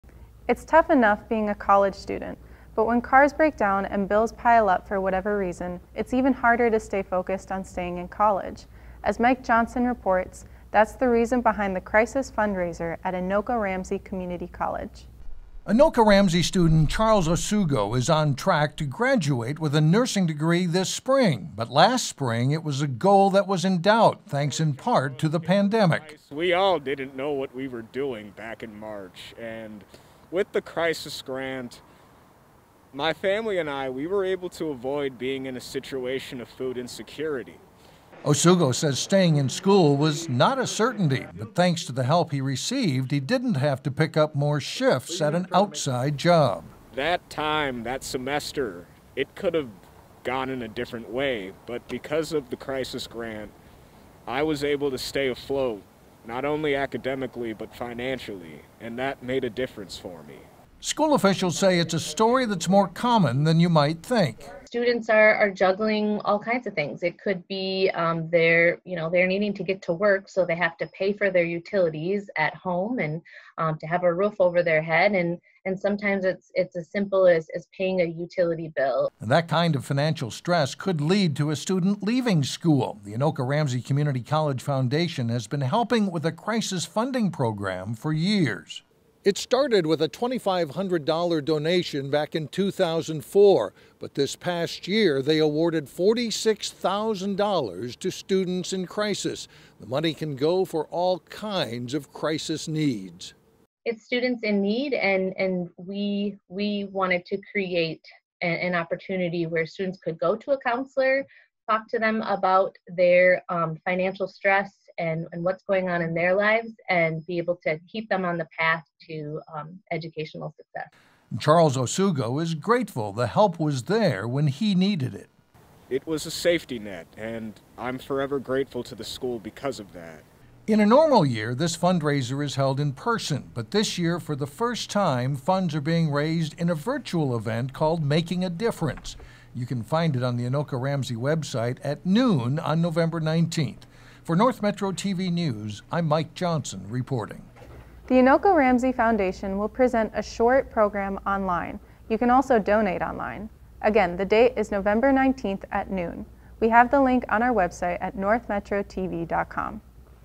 Anoka-Ramsey Community College Helps Students In Financial Need
School holding online fundraiser to help with support for those who may need a little boost during difficult times.